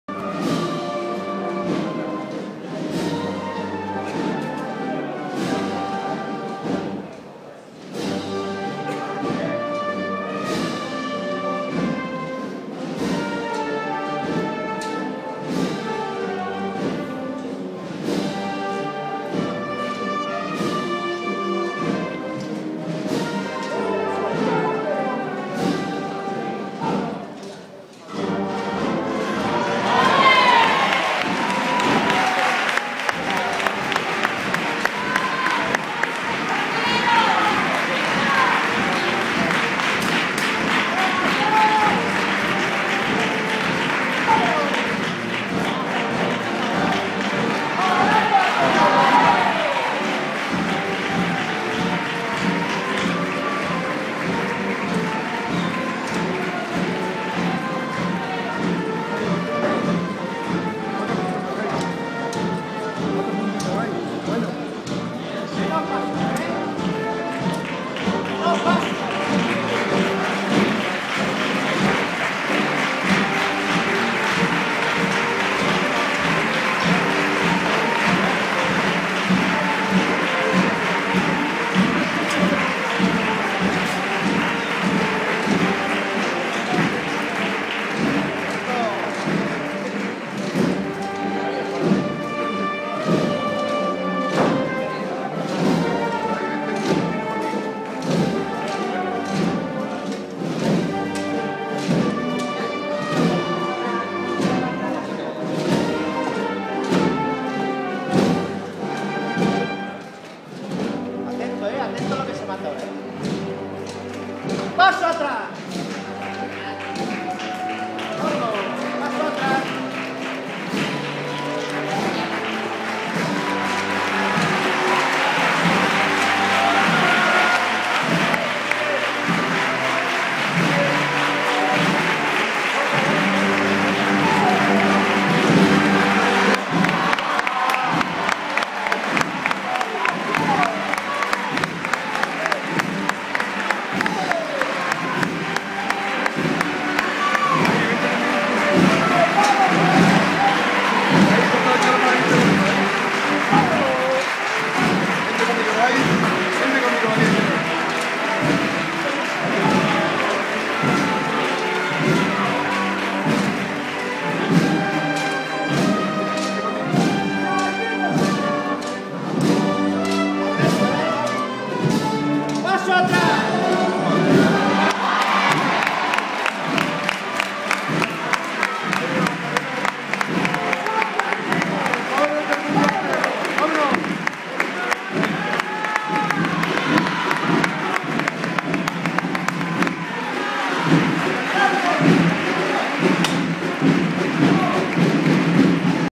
Virgen de la Macarena 2015 (Madrid)
Entrada de la Esperanza Macarena en la Colegiata de San Isidro.